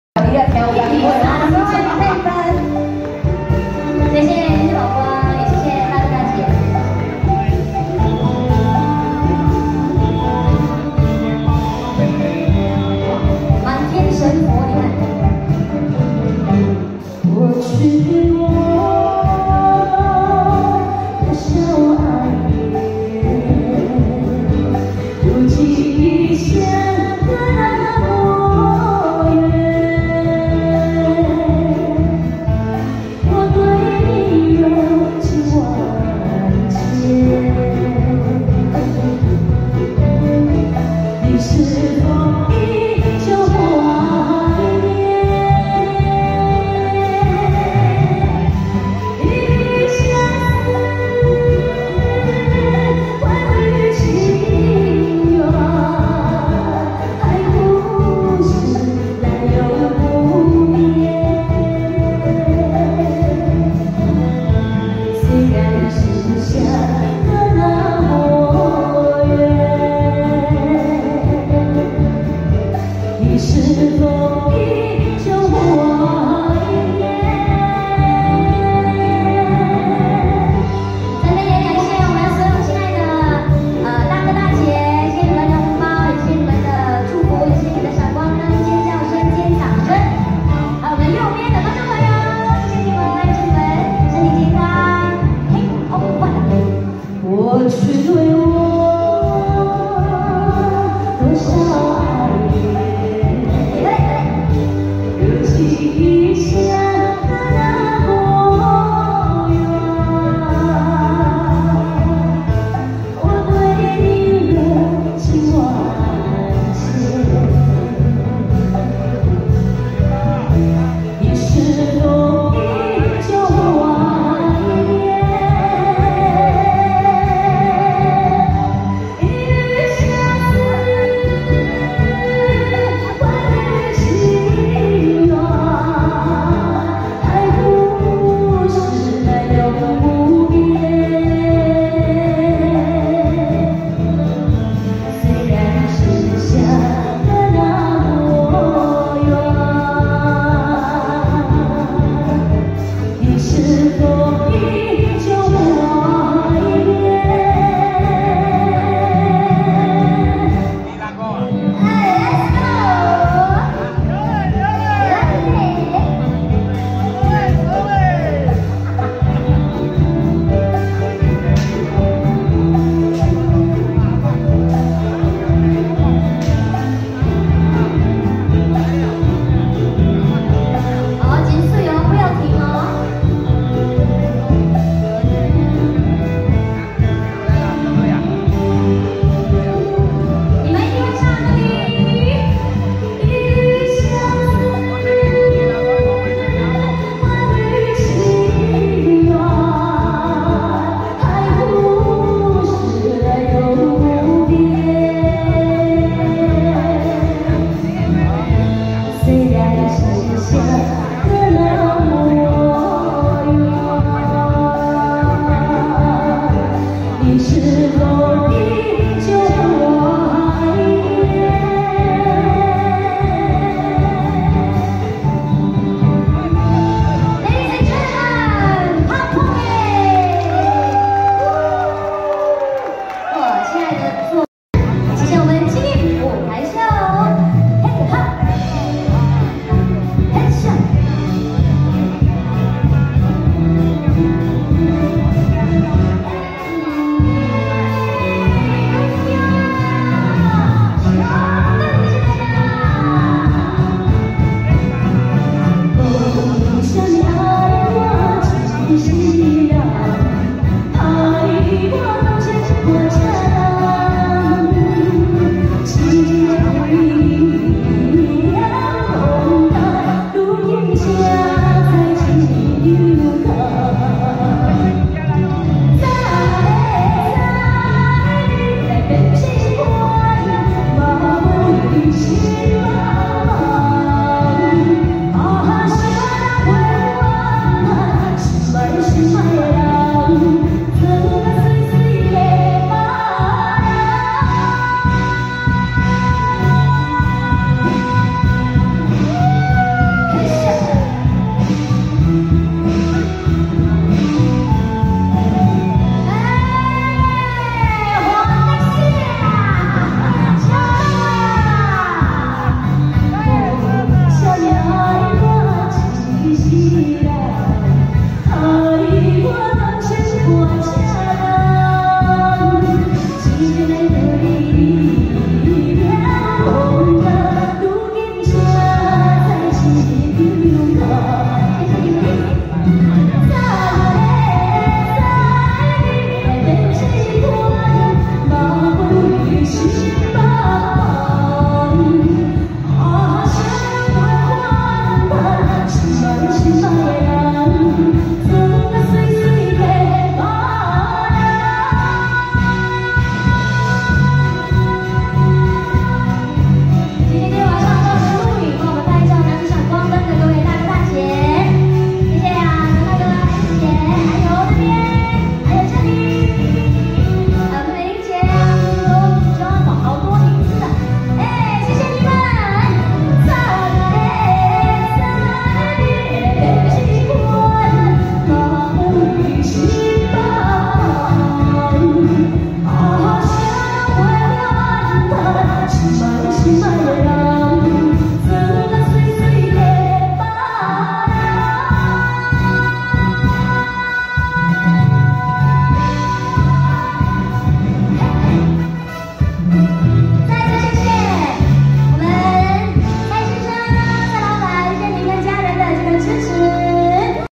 日期 : 28 December 2023 演唱歌手 : 李佩芬 演唱
演唱歌手 : 李佩芬  演唱

制作单位 : 精义娱乐制作
全场司仪 : 皓皓 , 宝贝姐妹
乐队伴奏 : 飞鹰大乐队
灯光音响 : 义峰舞台设计
主办单位 : 王府宫
地点 : Blk  293 Yishun Street 22